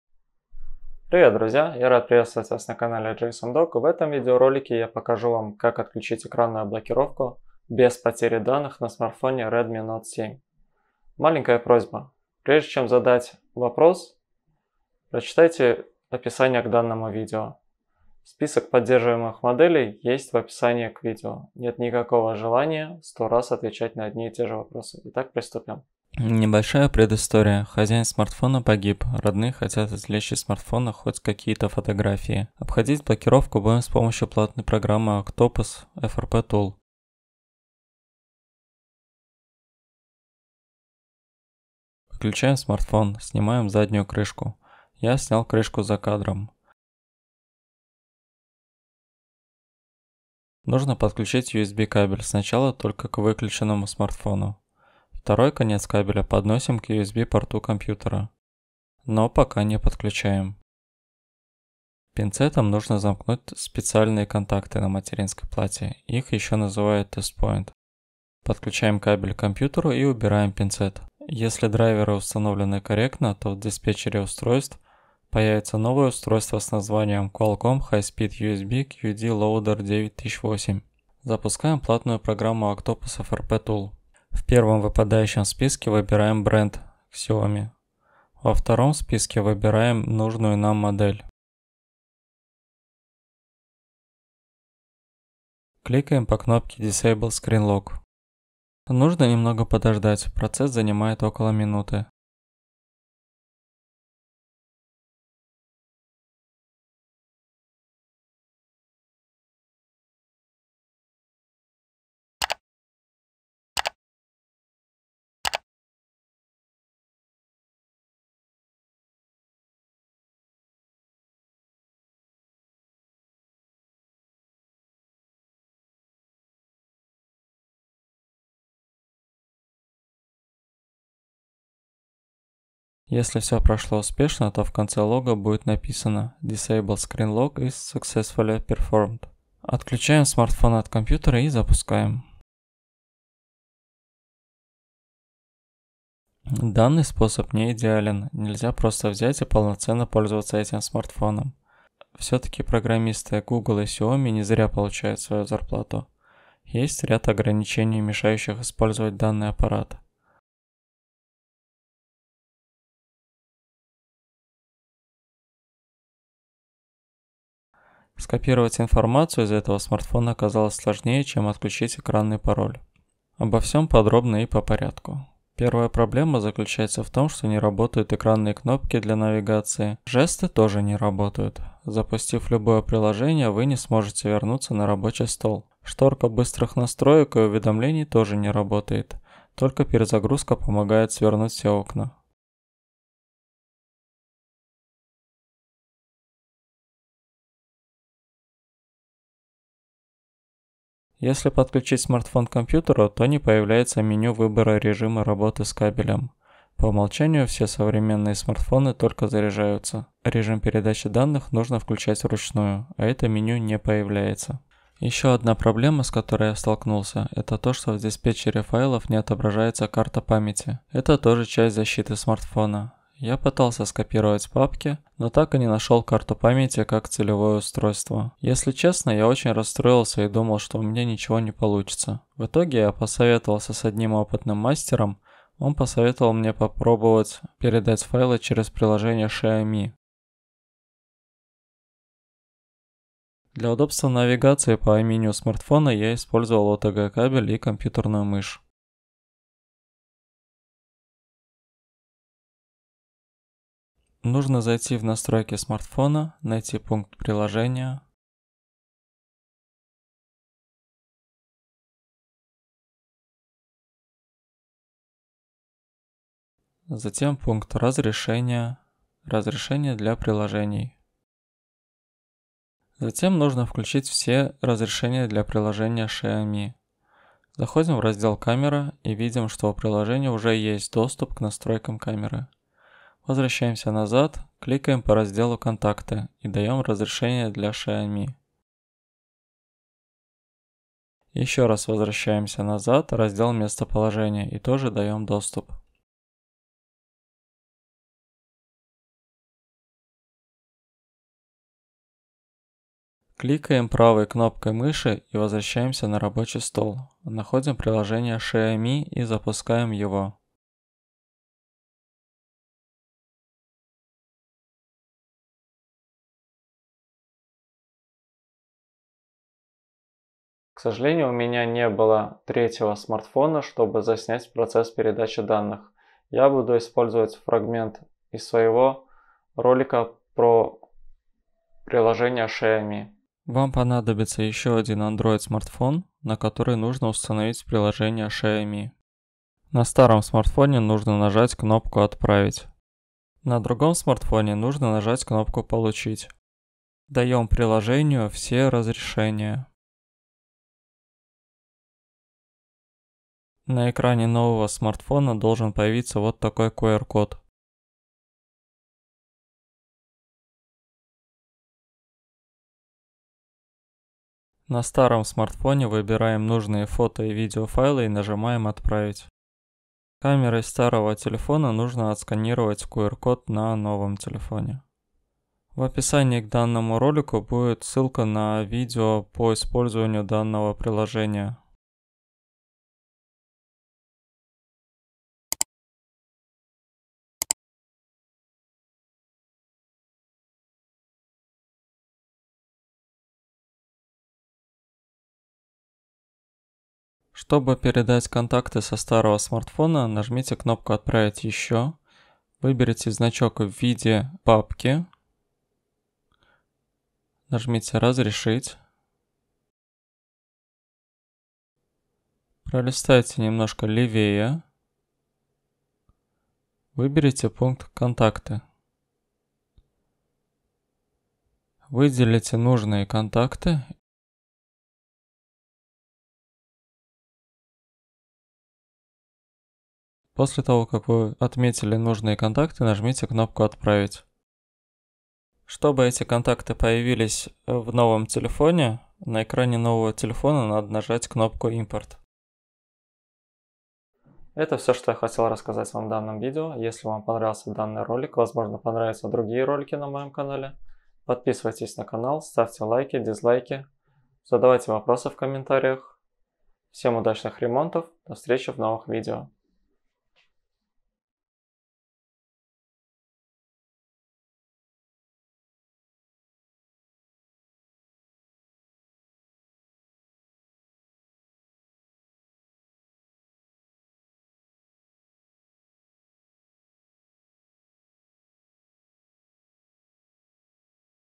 Redmi Note 7 обход пароля без потери данных
Привет, друзья! Я рад приветствовать вас на моём канале. В этом видеоролике я покажу вам как отключить экранную блокировку без потери данных на смартфоне Redmi note 7 с помощью платной программы Octoplus frp tool.
Программа поддерживает следующие модели:
REDMI GO
REDMI 4 PREMIUM EDITION
REDMI 4X
REDMI 5A
REDMI 5 PLUS
REDMI S2
REDMI Y1
REDMI Y2
REDMI NOTE 5
REDMI NOTE 5A
Redmi  NOTE 7
MI 6X
Список моделей возможно когда-нибудь пополниться. Как только будут новости, я добавлю эту модель в описание к этому ролику.
Вот другие полезные ролики по данной теме.